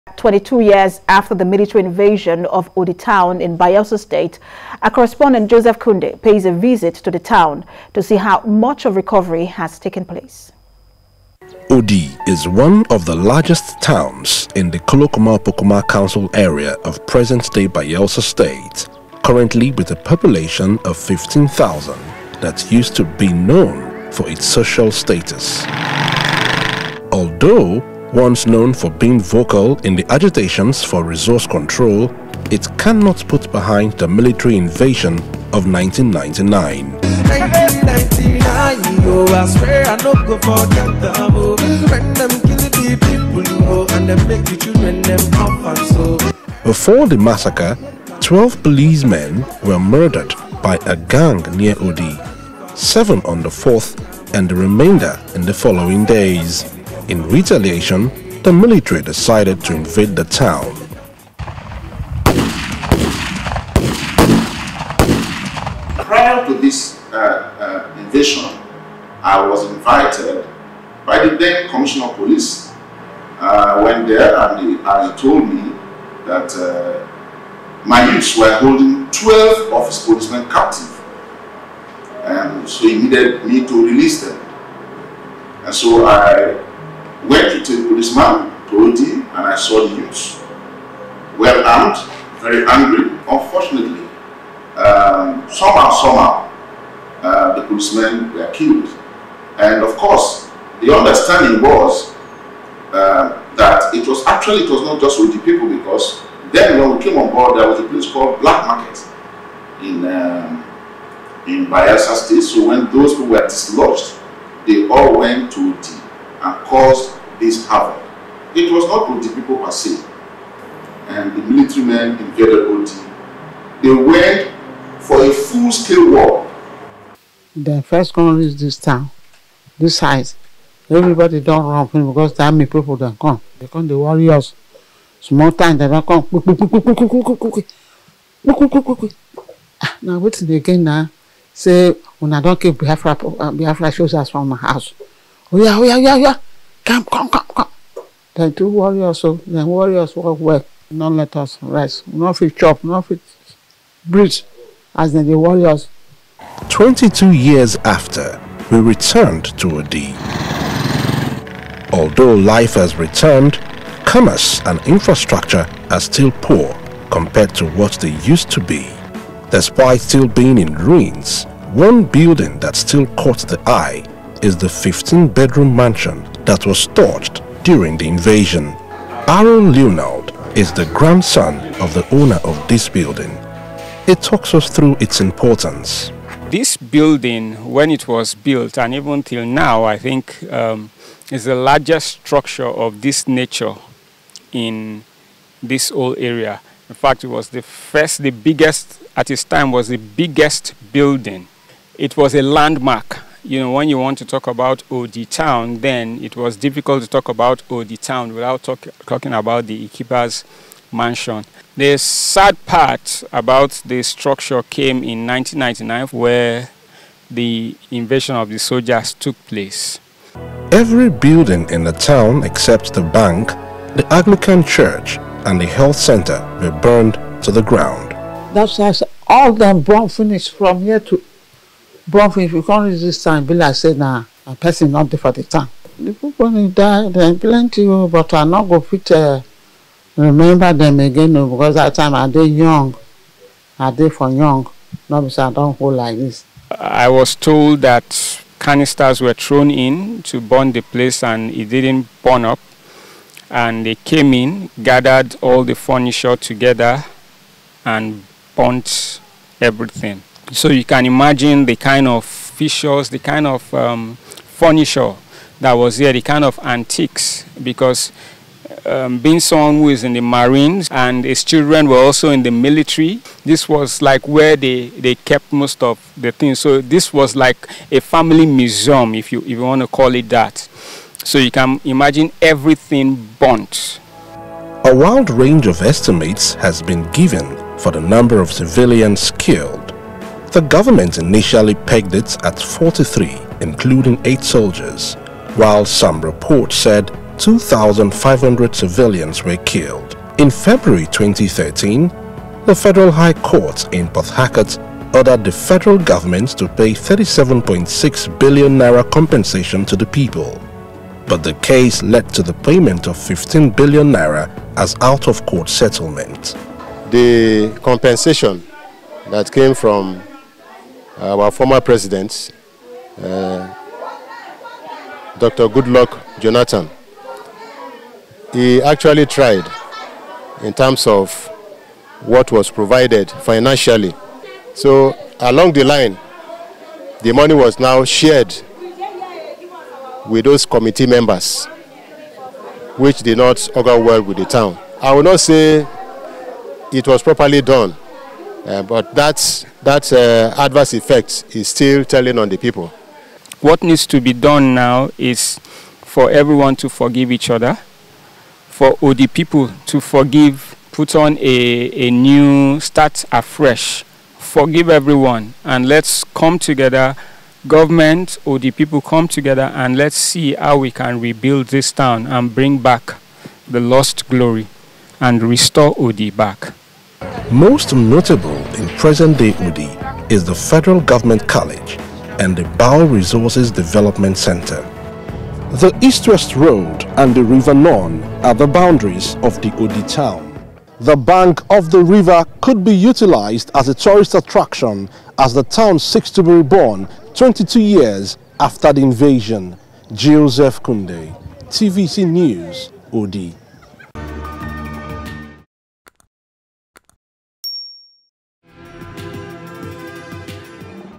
How Odi Town In Bayelsa State has Fared 22-Years After Military Invasion
Twenty-two years after the military invasion of Odi town in Bayelsa state, TVC News correspondent, Joseph Kunde pays a visit to the town, to see how much recovery has taken place.